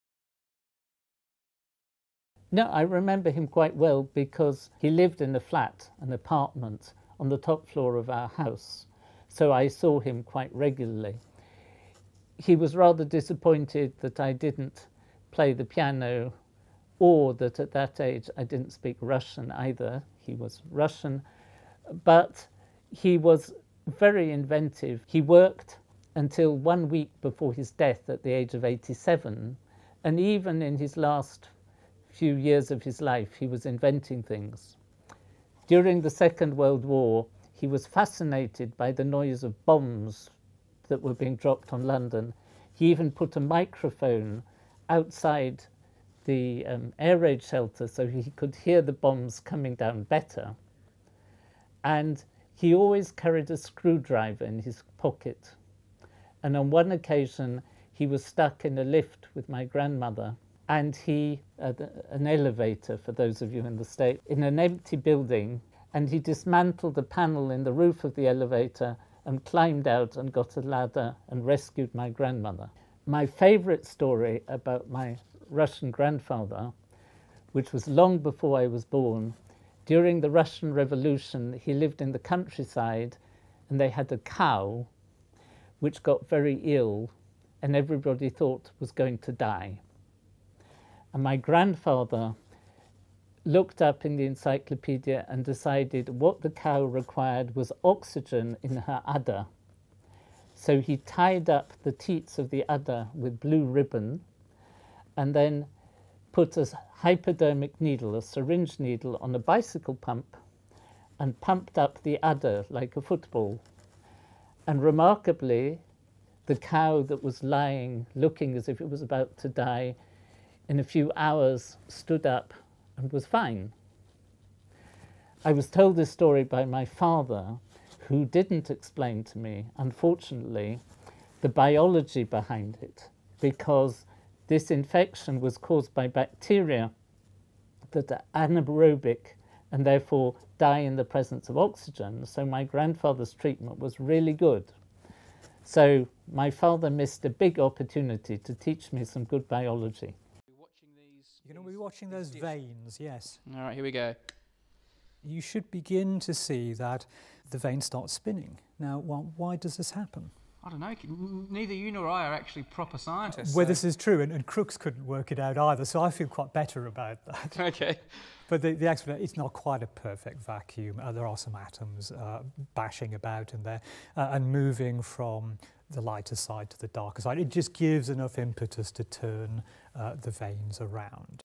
The Professor's Grandfather (Geissler Tubes extra footage)
Extra footage from our Geissler Tubes video on Periodic Videos. Here Professor Martyn Poliakoff discusses his grandfather Joseph.